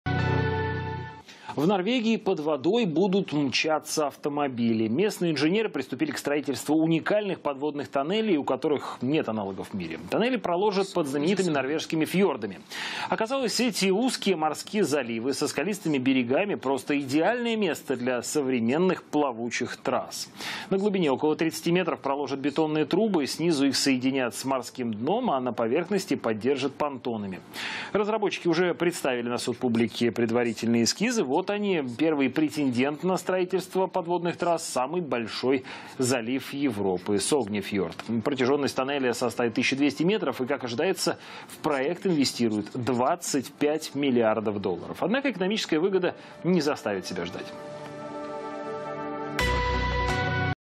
В Норвегии появятся подводные тоннели
У фьордских тонелей нет аналогов в мире